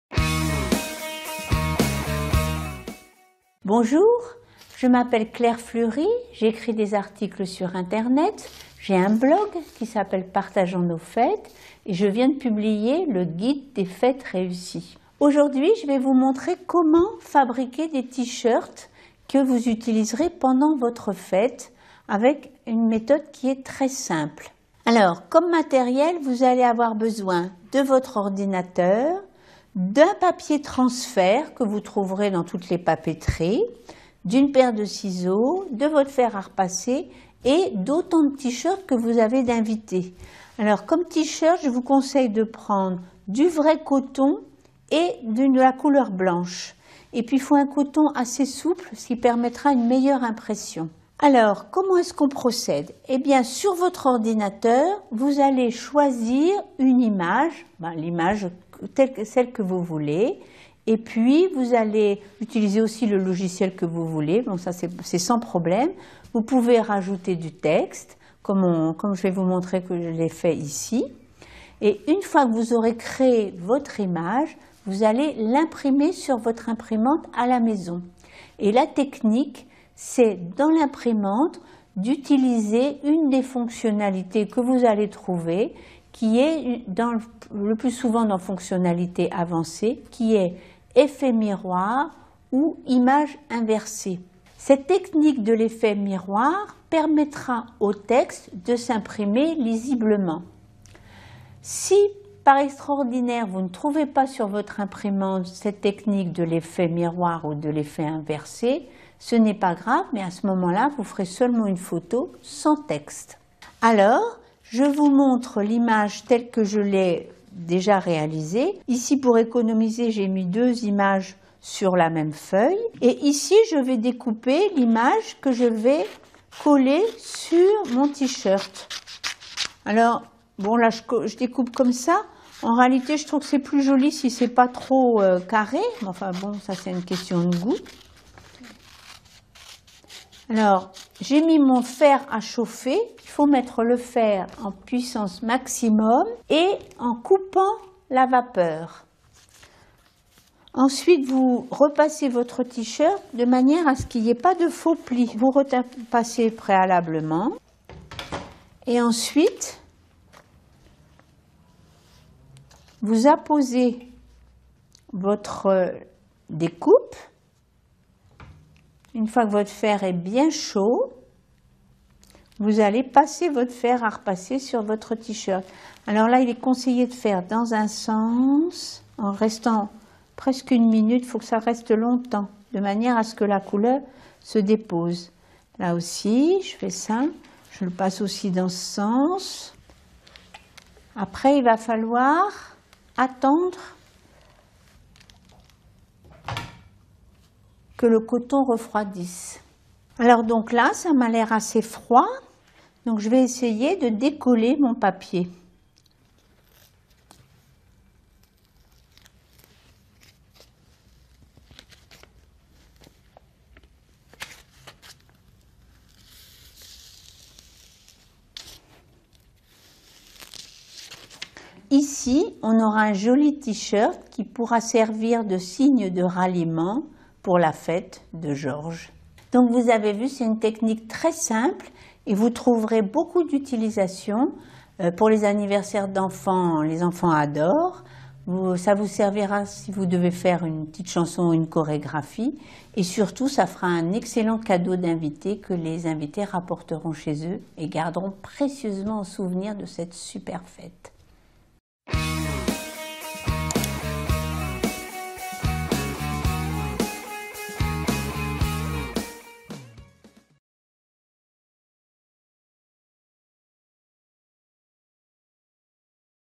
Imprimer des photos sur des T-shirts: mode d'emploi
Comment fabriquer des T-shirts personnalisés ? Suivez la vidéo .

Claire Flury, auteure du "Guide des fêtes réussies" et animatrice du blog nous explique étape par étape comment réaliser ces T-shirts.